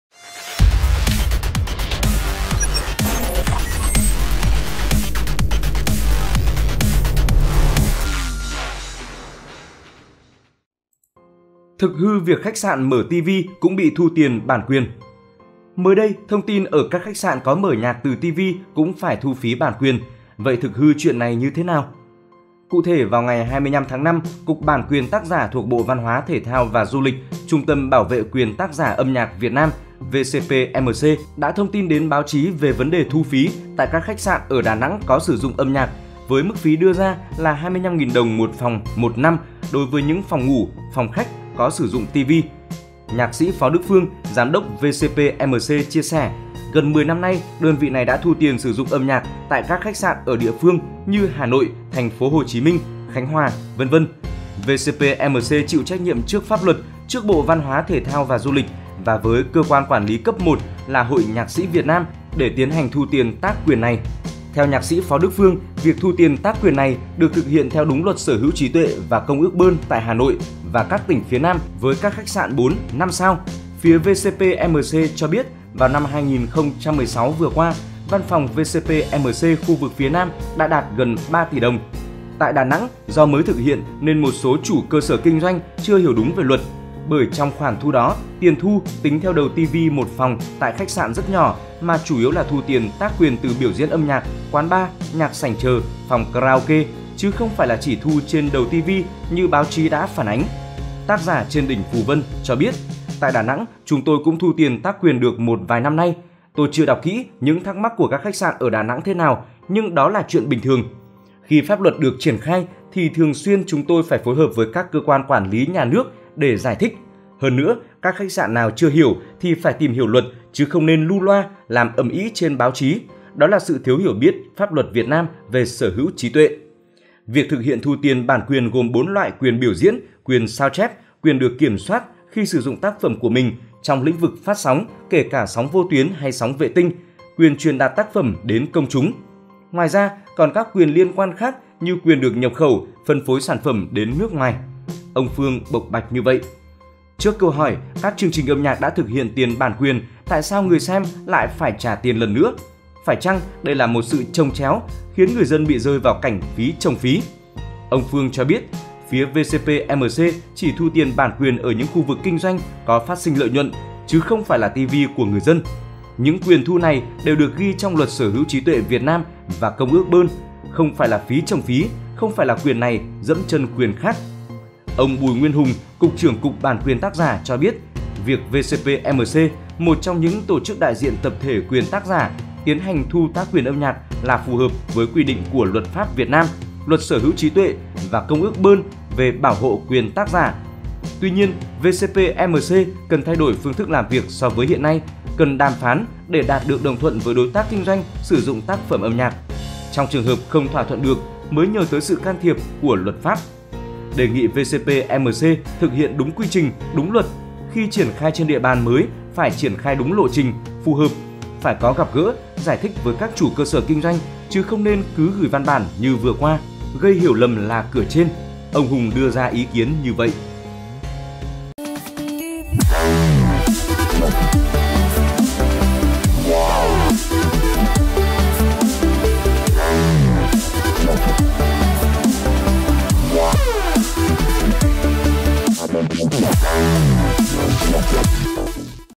Tin tức trong ngày - Thực hư việc khách sạn mở tivi cũng bị thu tiền bản quyền
Mới đây, thông tin ở các khách sạn có mở nhạc từ tivi cũng phải thu phí bản quyền. Vậy thực hư chuyện này như thế nào?